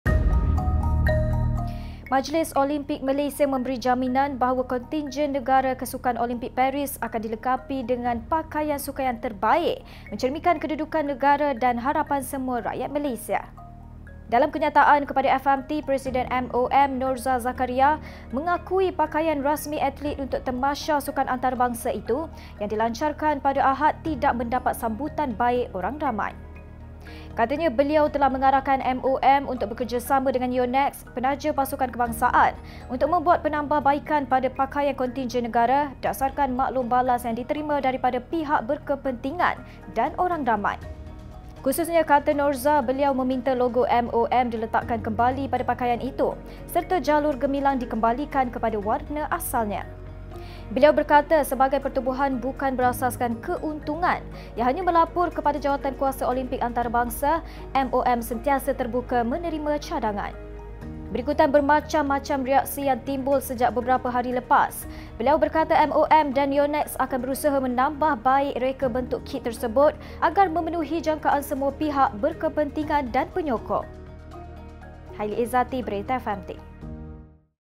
Kit Olimpik kontroversi akan ditambah baik, kata ketua MOM
Majlis Olimpik Malaysia (MOM) memberi jaminan bahawa kontinjen negara ke Sukan Olimpik Paris akan dilengkapi dengan pakaian sukan yang ‘terbaik mencerminkan kedudukan negara dan harapan semua rakyat Malaysia’.